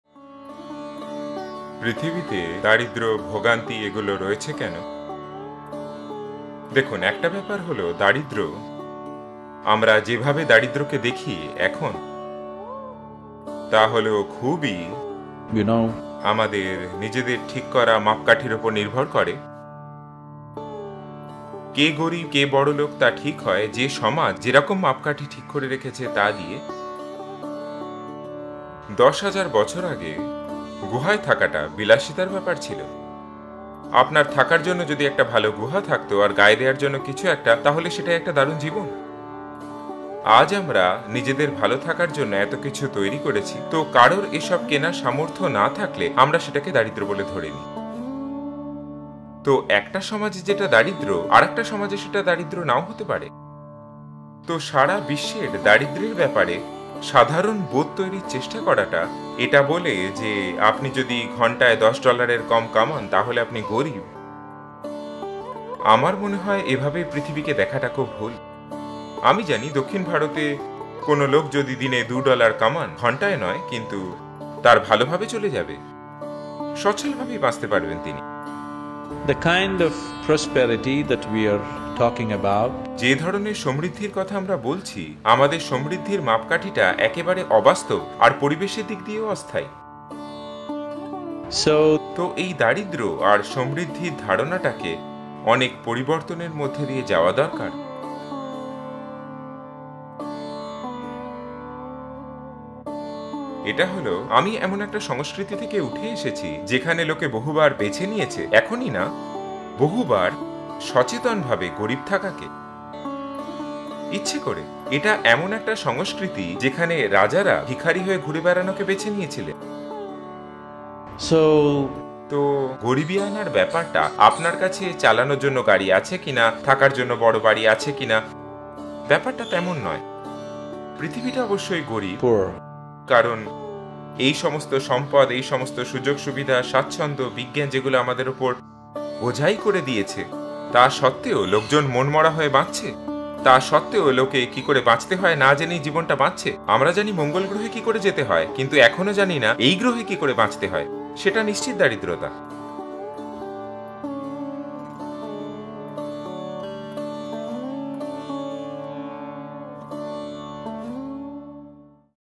নিজেকে গরিব বলে ভাবেন? ভুল ধারণা ! | Why poverty and suffering?
আপনি কি নিজেকে গরিব বলে ভাবেন? লোকে যা কিনছে তা কেনার সামর্থ্য নেই বলে মন খারাপ করেন? হতেও তো পারে আপনার ধারণা ভুল! আসুন আজকের ভিডিওতে শুনে নিই সদগুরু কি বলছেন...